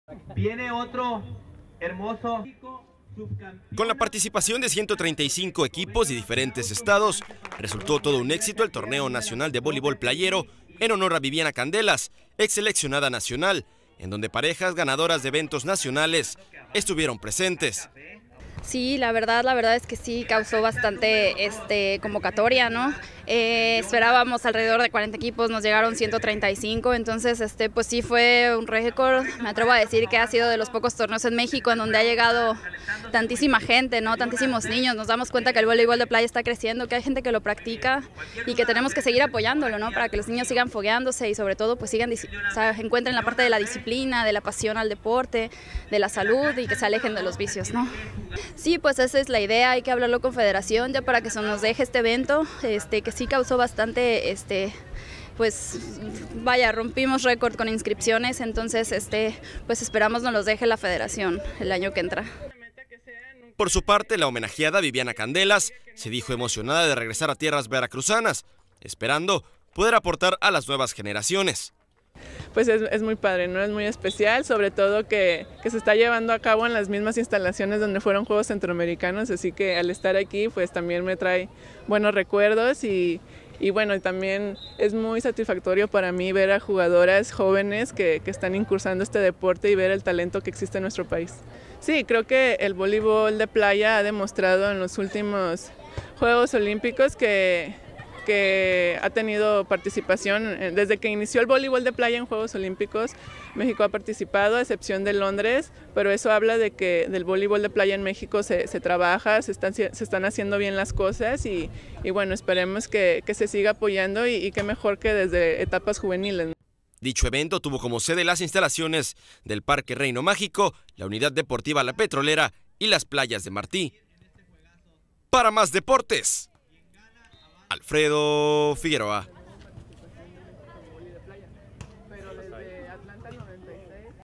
Primera edición del torneo de voleibol de playa "Bibiana Candelas"
TODO UN ÉXITO LA PRIMERA EDICIÓN DEL TORNEO DE VOLEIBOL DE PLAYA “BIBIANA CANDELAS”